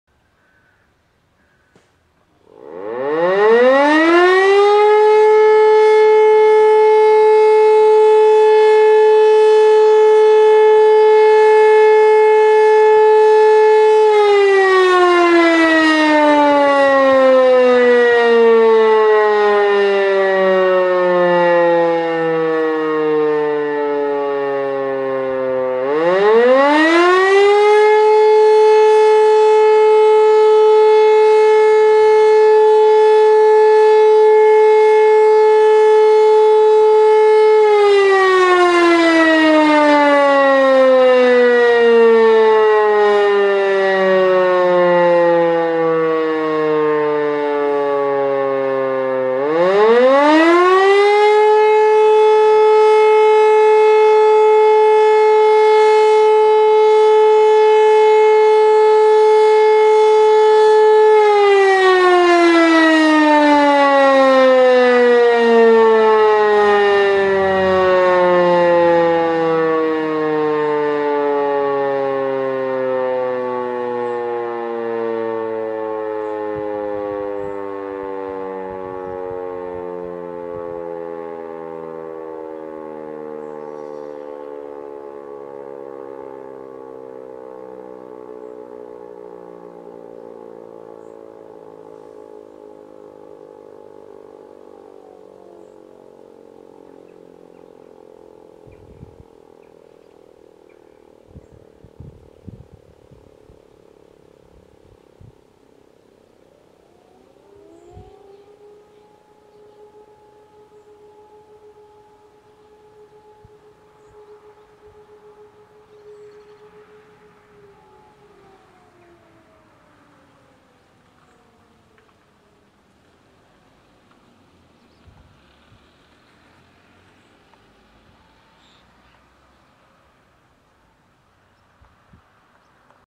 Monatliche Probe einer Hochmastsirene Typ E57 - Sirenenprobe Lahausen
Monatliche Überprüfung der E57-Sirenenanlagen der Ortschaft Lahausen und der Gemeinde Weyhe. Zu sehen und zu hören ist im Vordergrund eine Sirene als Hochmastmontage mit dem Signal Feueralarm.

Entfernung zur E57: ca. 40-50 m
Signal: Feueralarm